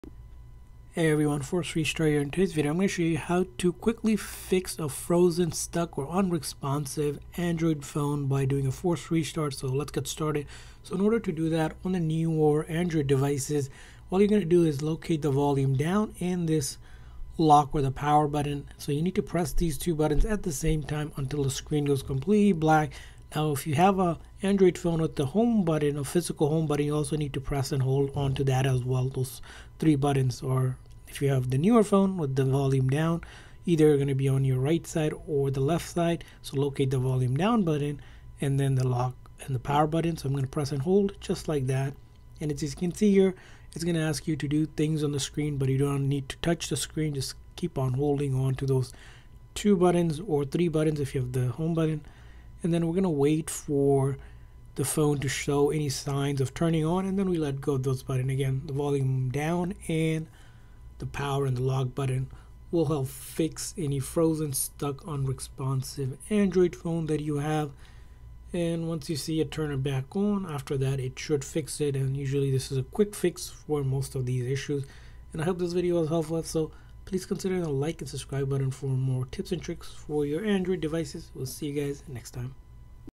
How to Force Restart Frozen or Stuck Android Phone
If your Android phone is frozen, stuck or unresponsive, you can perform a force restart to fix it quickly. In this video I show you how to force restart a frozen or stuck screen Android phone.